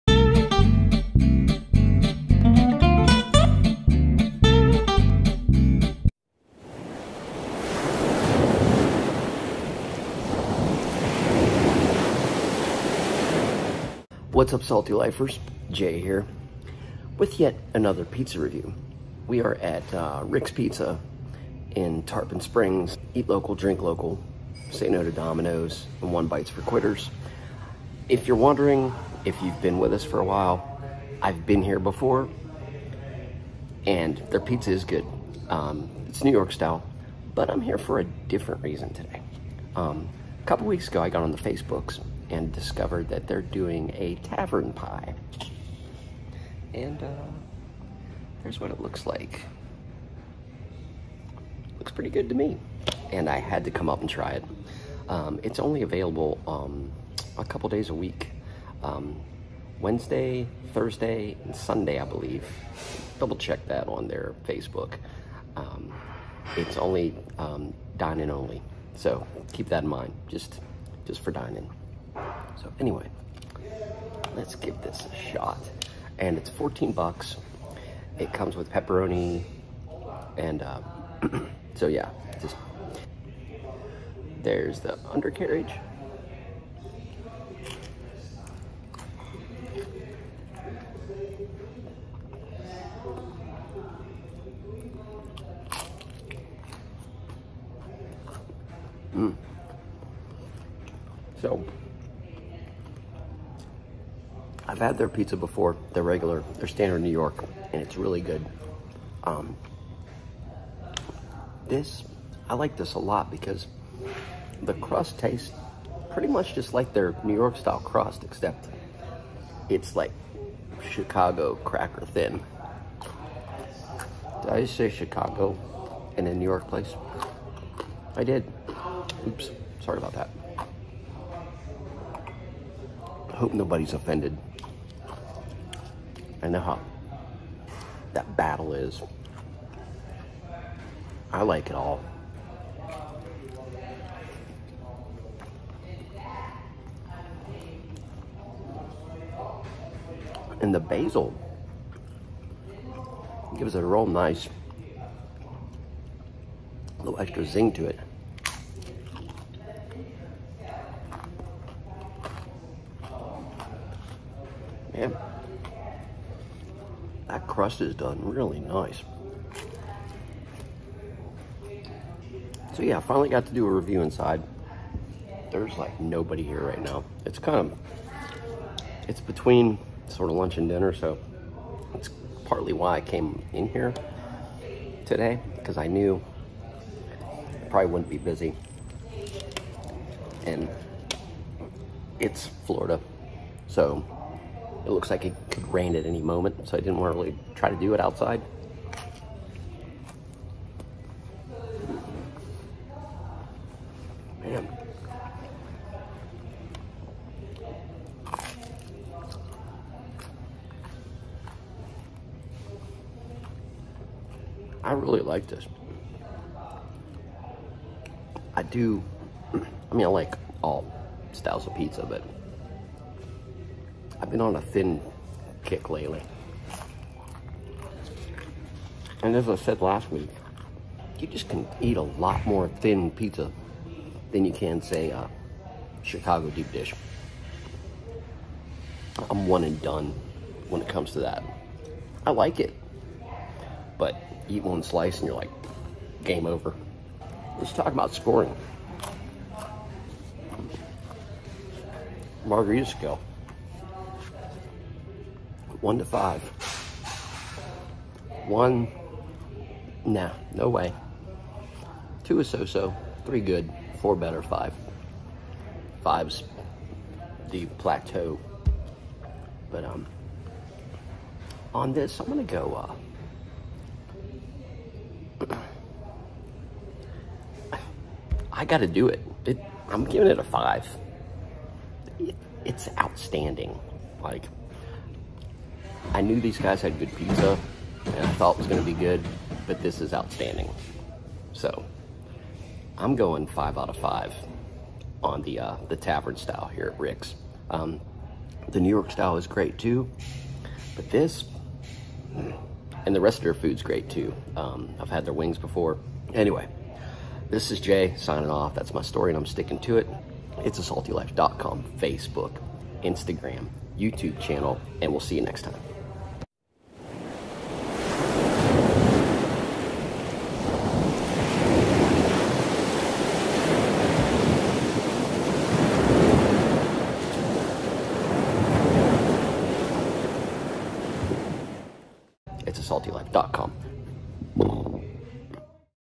Rick's New York Pizza of Tarpon Springs Episode #72
Pizza Review Episode #72. Ricks New York Pizza of Tarpon Springs Florida! Ricks is located at 905 Klosterman Rd, Tarpon Springs. In this episode I try out the brand new Tavern Style Pizza.At first it was only available on a limited basis but now it's available all day every day. The crust was thin and done perfectly. The topping ratio was spot on. This pizza was fantastic and I highly recommend checking to out. I've been to Rick's before and had the standard NY style they offer and I'll repost that in the comments.. If you enjoy food, fun and travel please subscribe to our channel @itsasaltylife